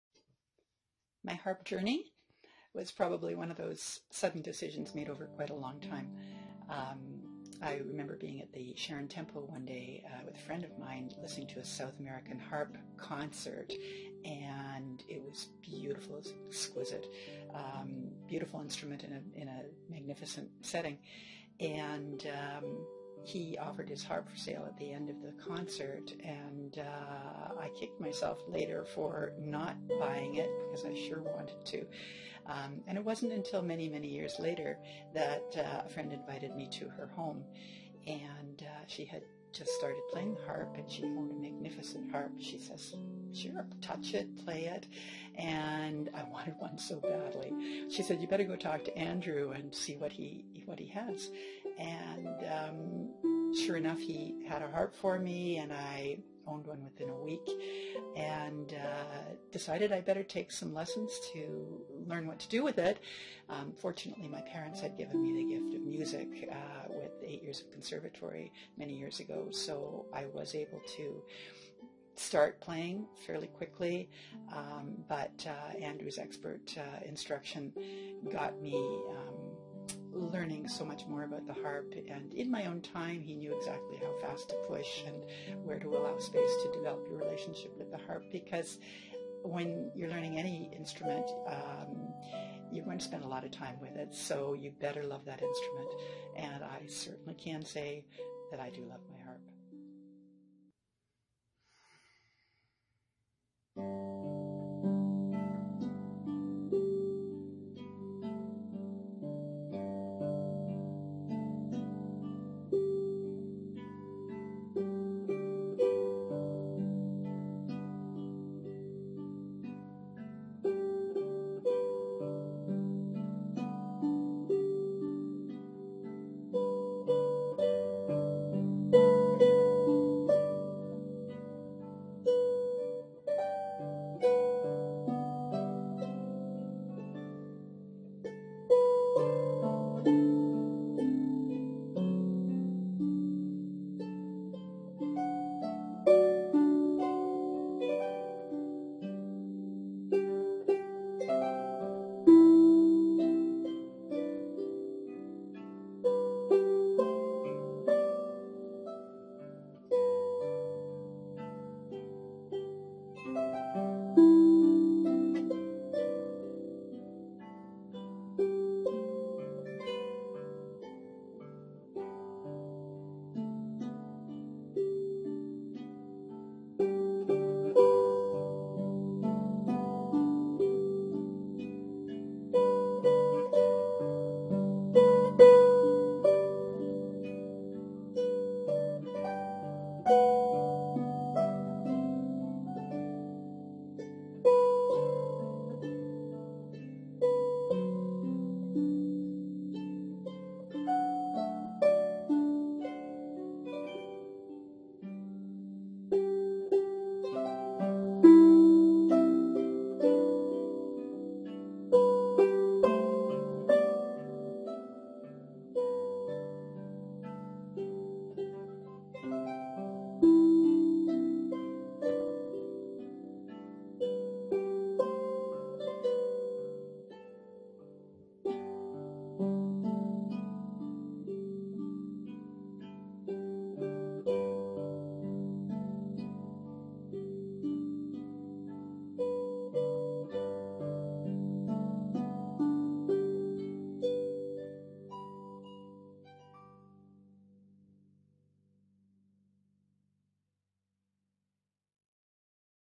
Spotlight on Adult Harp Class at Harps on the Hill: Anne Shelton
Student Showcase: Anne, shares her love story with the harp.